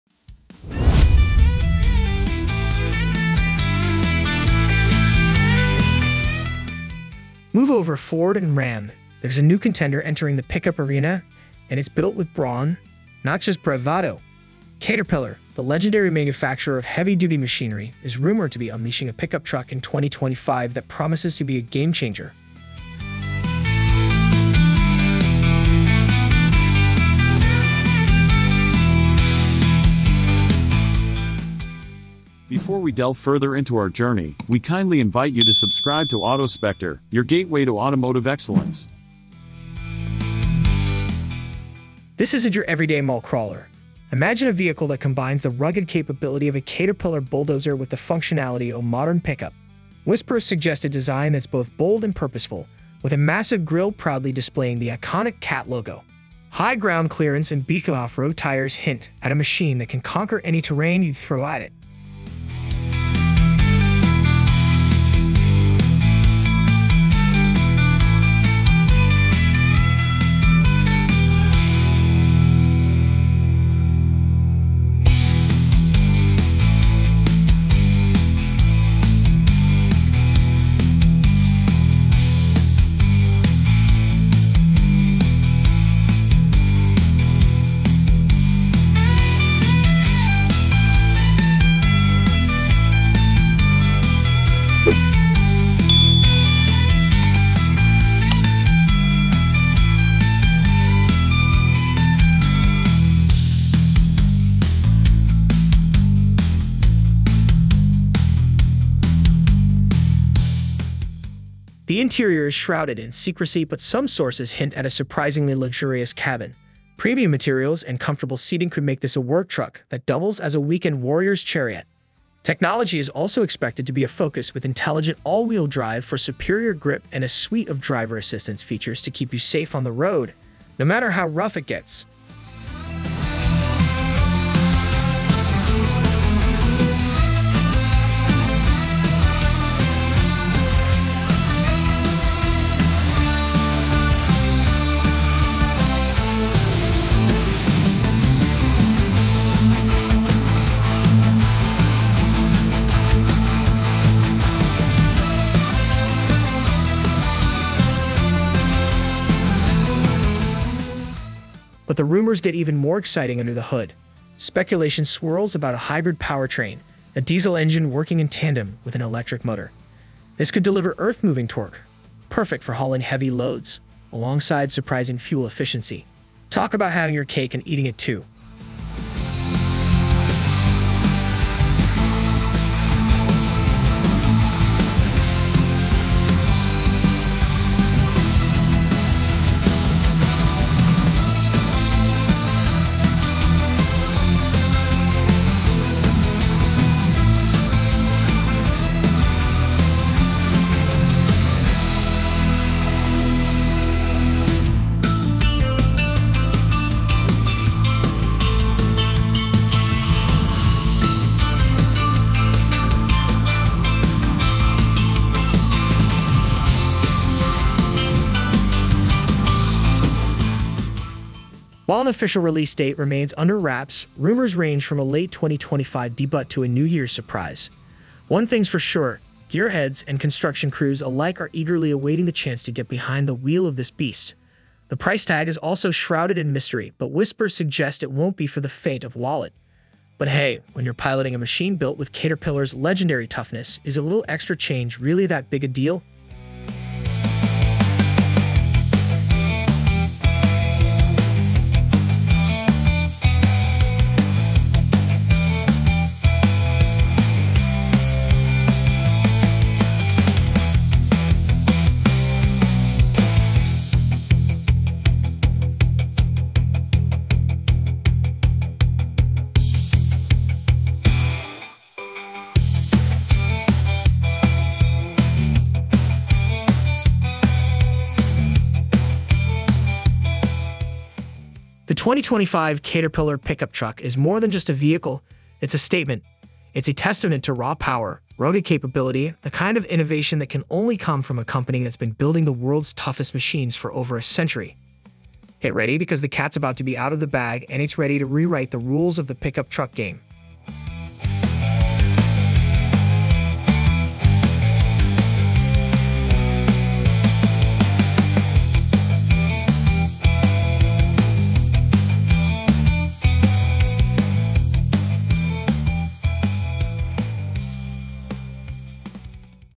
2025 Caterpillar Pickup Truck: The Beast is Roaring to Life
Move over, Ford and Ram, there's a new contender entering the pickup arena, and it's built with brawn, not just bravado. Caterpillar, the legendary manufacturer of heavy-duty machinery, is rumored to be unleashing a pickup truck in 2025 that promises to be a game-changer.

This isn't your everyday mall crawler. Imagine a vehicle that combines the rugged capability of a Caterpillar bulldozer with the functionality of a modern pickup. Whispers suggest a design that's both bold and purposeful, with a massive grille proudly displaying the iconic Cat logo. High ground clearance and beefy off-road tires hint at a machine that can conquer any terrain you throw at it.

But the rumors get even more exciting under the hood. Speculation swirls about a hybrid powertrain, a diesel engine working in tandem with an electric motor. This could deliver earth-moving torque – perfect for hauling heavy loads –  alongside surprising fuel efficiency. Talk about having your cake and eating it too!

The interior is shrouded in secrecy, but some sources hint at a surprisingly luxurious cabin. Premium materials and comfortable seating could make this a work truck that doubles as a weekend warrior's chariot. Technology is also expected to be a focus, with intelligent all-wheel drive for superior grip and a suite of driver-assistance features to keep you safe on the road, no matter how rough it gets.

While an official release date remains under wraps, rumors range from a late 2025 debut to a New Year's surprise. One thing's for sure: gearheads and construction crews alike are eagerly awaiting the chance to get behind the wheel of this beast. The price tag is also shrouded in mystery, but whispers suggest it won't be for the faint of wallet. But hey, when you're piloting a machine built with Caterpillar's legendary toughness, is a little extra change really that big a deal?

The 2025 Caterpillar Pickup Truck is more than just a vehicle; it's a statement. It's a testament to raw power, rugged capability, and the kind of innovation that can only come from a company that's been building the world's toughest machines for over a century.  Get ready, because the Cat's about to be out of the bag, and it's ready to rewrite the rules of the pickup truck game.